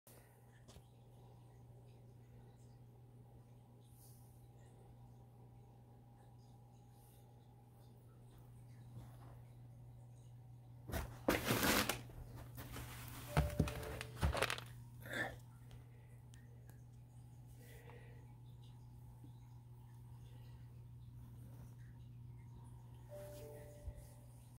New Equipment Python Gravel Cleaner and Tetra Min Flake ibulk unboxing (2022)
New Equipment Python Gravel Cleaner and Tetra Min Flake in bulk unboxing.
3 Comments